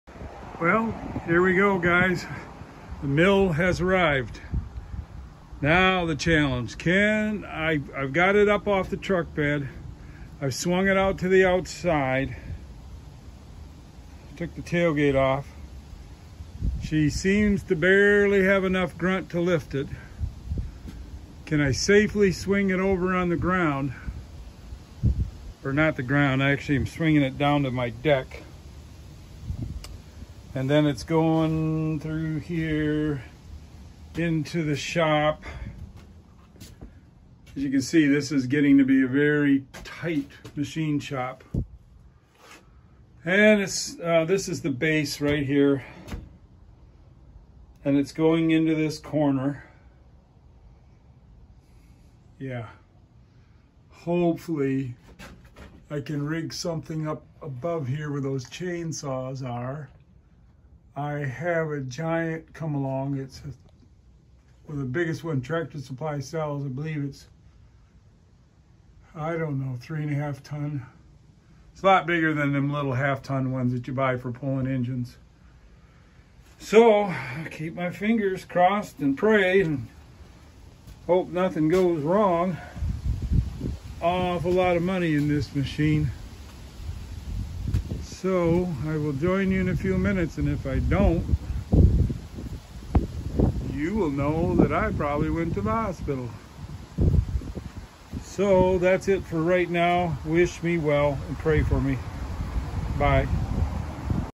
New Precision Matthews 727V mill Part 1.
Unloading a heavy new milling machine with my Harbor Freight pickup truck cran